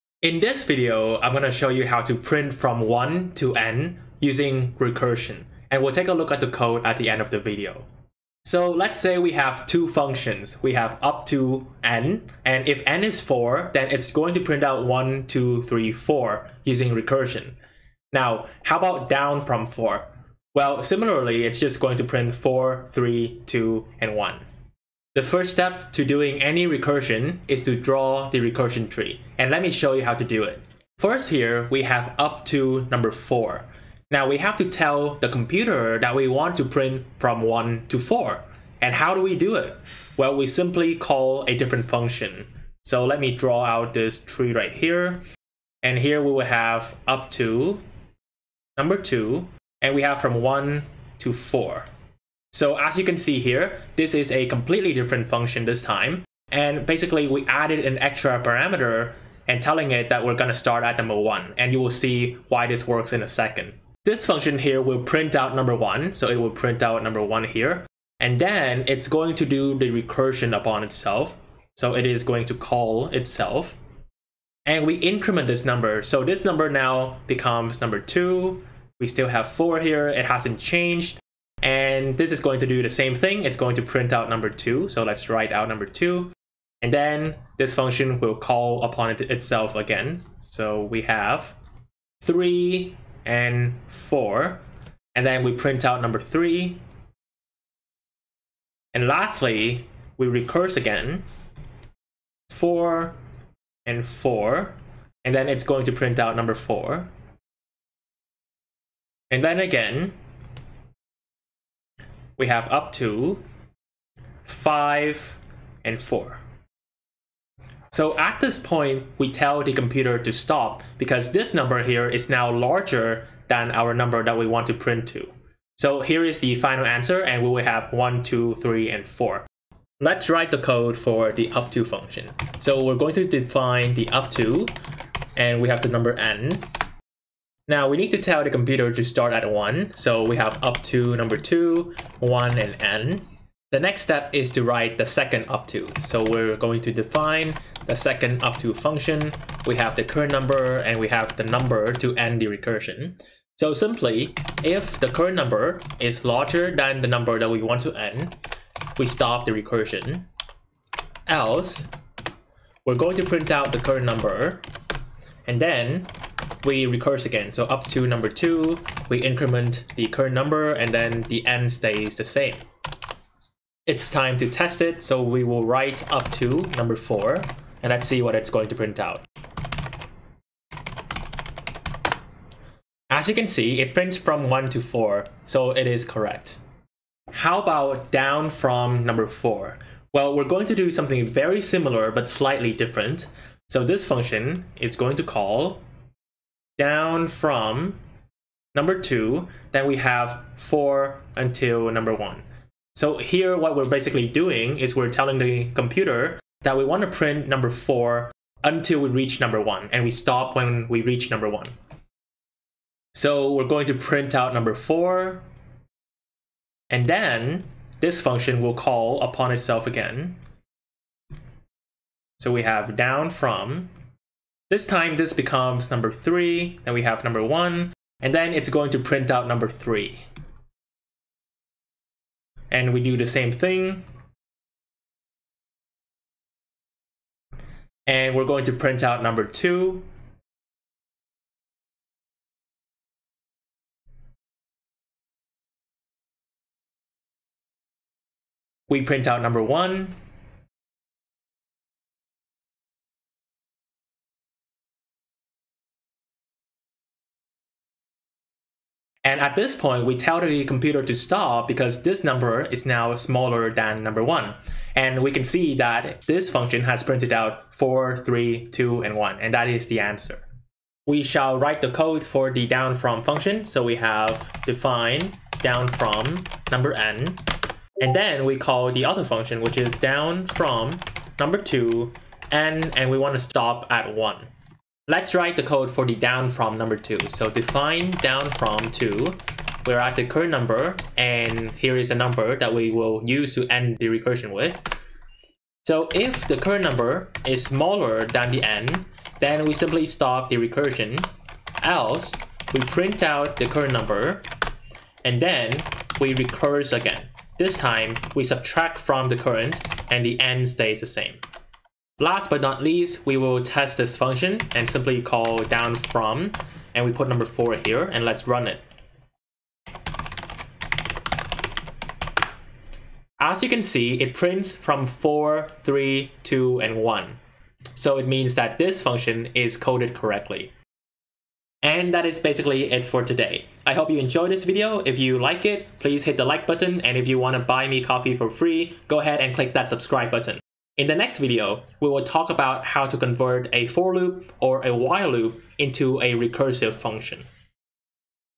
Print 1 To N Recursively (Code Provided)
In this video, I will explain how to print from 1 to N and N to 1 recursively and I will also explain the Python code at the end of the video.

What is recursion? Recursion is a function that calls upon itself. Here, we write a function that calls upon itself and print natural numbers from 1 to a given number N and vice versa. While it is easy to use a for loop or while loop to do this, it is important that you understand these recursion steps to have a strong foundation to learn more complex recursive algorithm in the future.

In the next video, I will explain how to convert a for loop and while loop into a recursive function. Why is this important? Sometimes you might want to write a recursive function that does the same thing as a for loop, and I will show you how to do this in the next video.

MY EQUIPMENT:
✅Camera: Samsung Galaxy J7